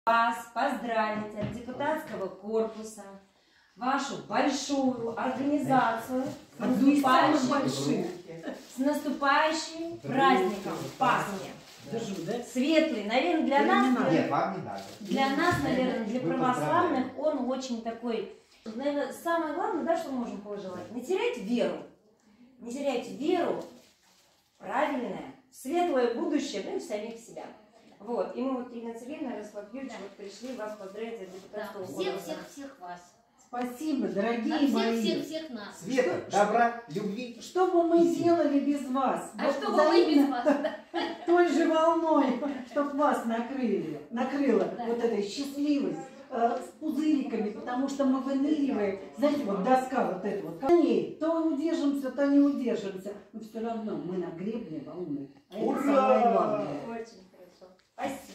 Депутаты поздравили Совет ветеранов с Пасхой
Накануне светлого праздника Пасхи в районный Совет ветеранов пришли гости. Глава муниципального округа Бирюлево Западное Елена Леликова, ее коллега по депутатскому корпусу Ярослав Бурых и руководитель аппарата Совета депутатов Татьяна Худова поздравили председателя объединения Наталью Фурман и в ее лице — всех активистов этой большой и дружной общественной организации.

Вручив пасхальные куличи, представители местного самоуправления пожелали Наталье Владимировне здоровья, достатка в доме, душевного мира и благословенных успехов в праведных трудах.